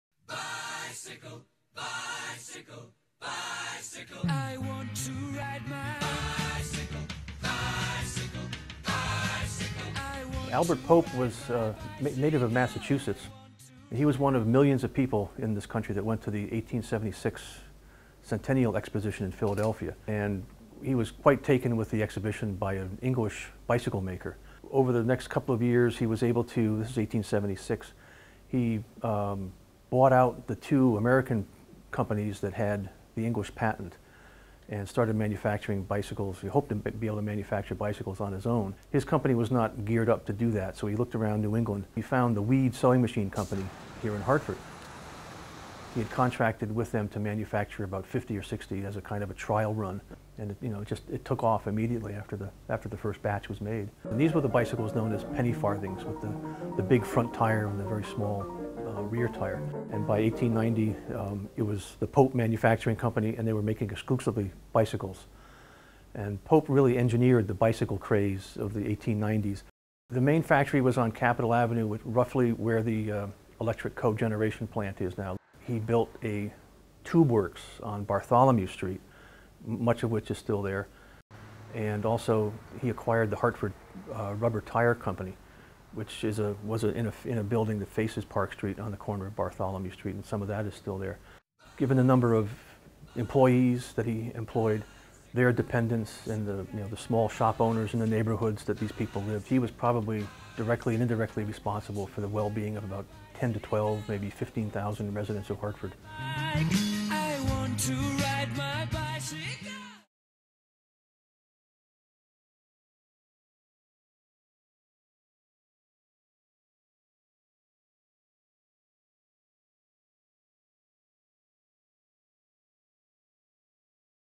Hidden History of the Bicycle
The Bicycle was first manufactured in the U.S. in Hartford, CT, by Pope Manufacturing.